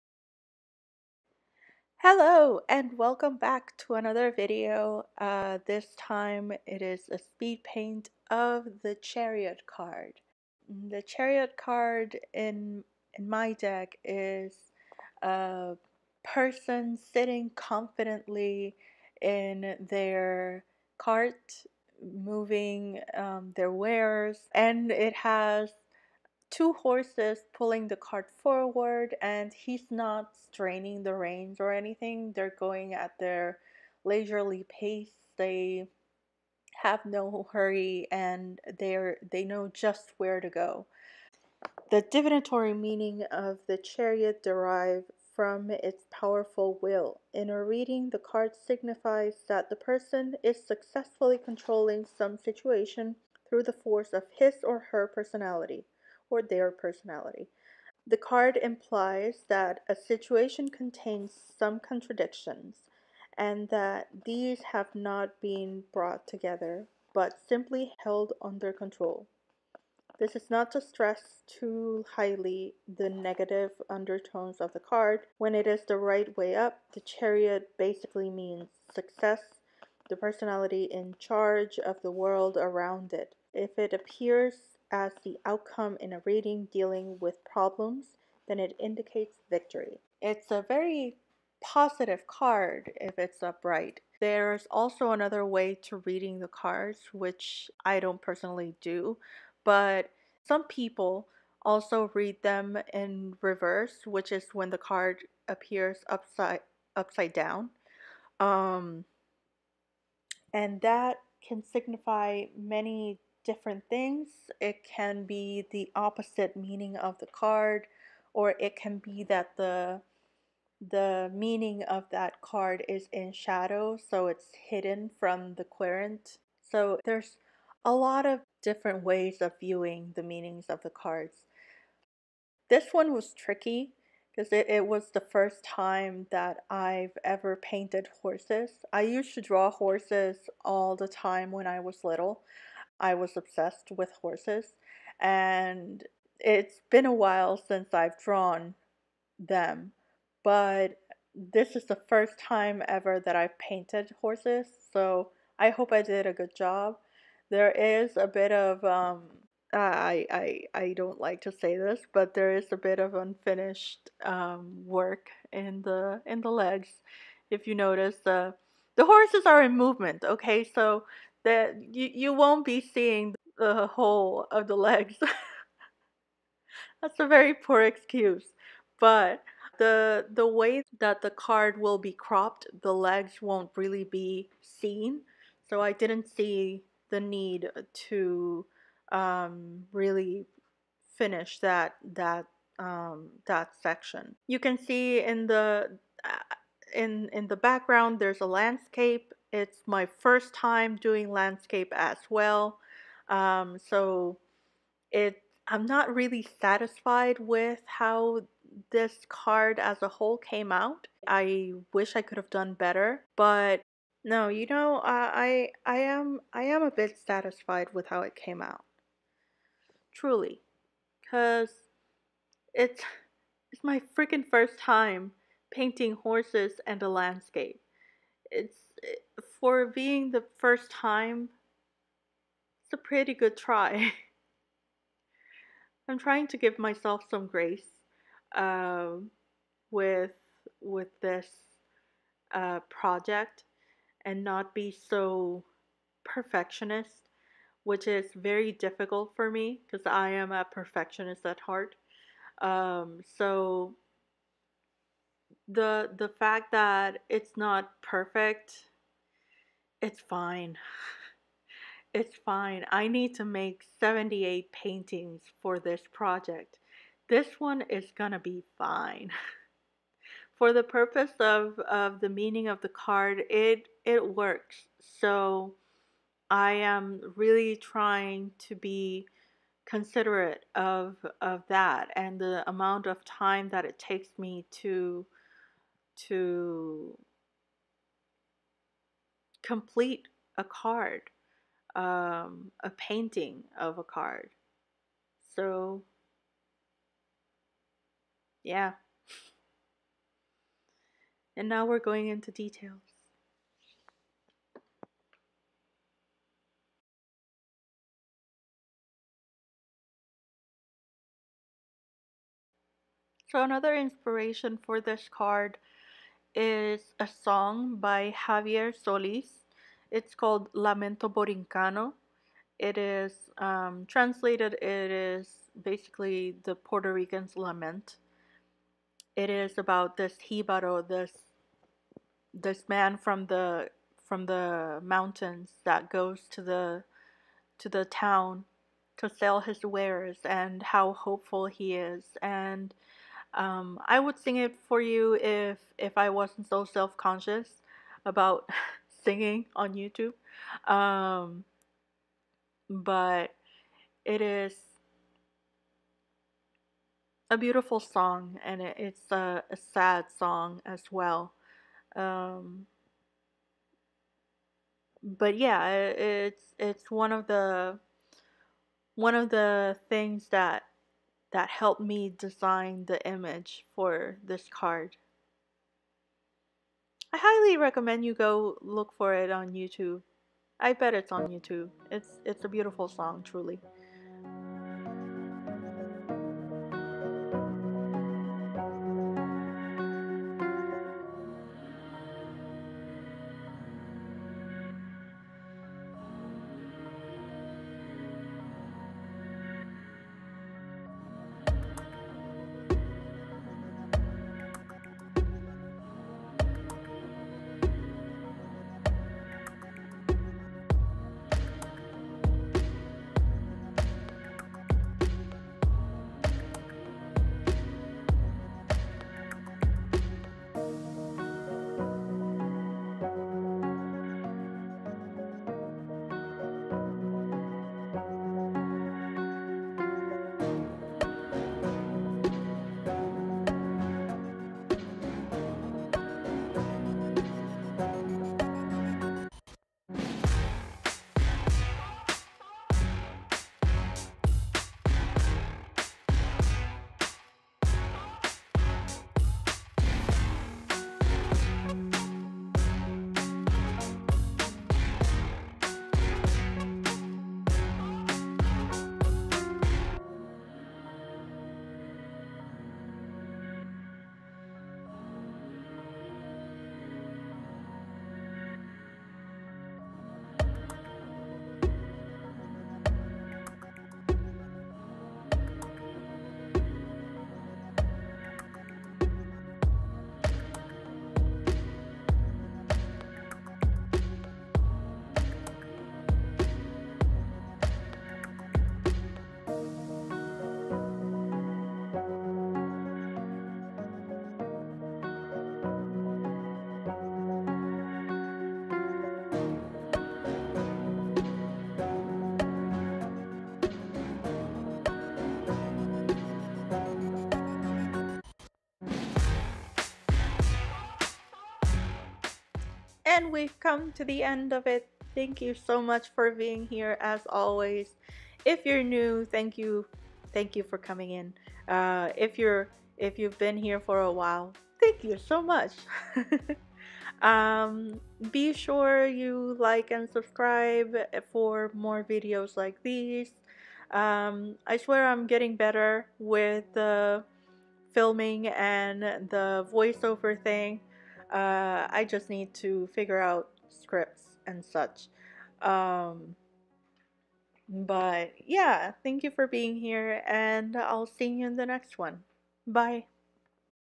Painting a Tarot Deck | The Chariot
Hello there, my name is Cristina and I am an artist.

This is the painting for The Chariot for my new tarot deck.